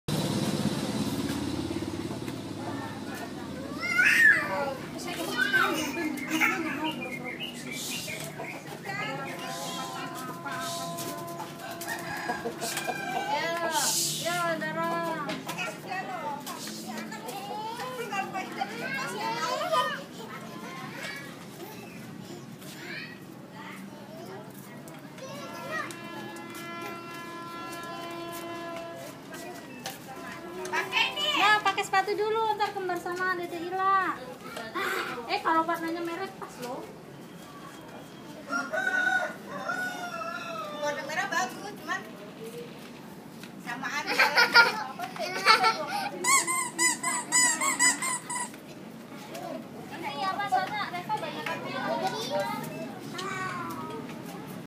refania naik sepeda roda tiga
refania safa calista jalan jalan di cicentang rawabuntu